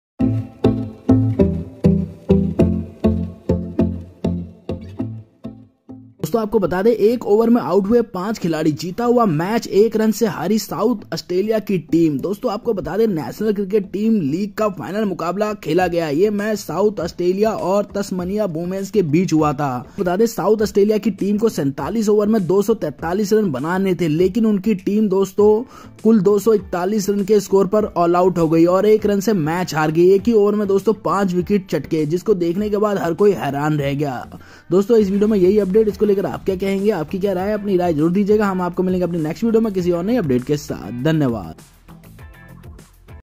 South Australia Team Lost 5 Wicket In Last Over, South Australia and Tasmania Women's team😱
Video Highlights ::
Cricket is a game of uncertainties and anything can happen at any time.  A video proving this is becoming increasingly viral on social media.  Actually, the final match of the Women's National Cricket League was played on last Saturday (February 25).  The match took place between South Australia and Tasmania Women's team, which was won by Tasmania by 1 run in the last over.
But here luck played its game.  COYTE was doing it over for Tasmania.  He dismissed the well set Annie O'Neil on the first ball of the over.  From here the match started turning.  Australia got one run on the next ball, but after that 4 wickets fell in 4 consecutive balls.  During this, 2 players returned to the pavilion after being run out, one lbw and one stumped behind the wicket.  Overall, 5 wickets fell in 6 balls and Tasmania won the match by 1 run.
 It is worth noting that till the last over of the match, victory was visible in Australia's bag, but inside the last 6 balls such incidents happened which surprised everyone.  Actually, South Australia needed only 4 runs to win the match in the last over.  Means 4 runs in 6 balls and the match would have been in the name of Australia.Significantly, the video of this incident is becoming increasingly viral on social media.  Talking about this match, South Australia Scorpions won the toss and elected to bowl first, after which Tasmania scored 264 runs in 50 overs on the basis of innings of Elyse Villani (110) and Naomi Stalenberg (75).  In the rain interrupted match, South Australia had to score 243 runs in 47 overs, but their team was all out for a total of 241.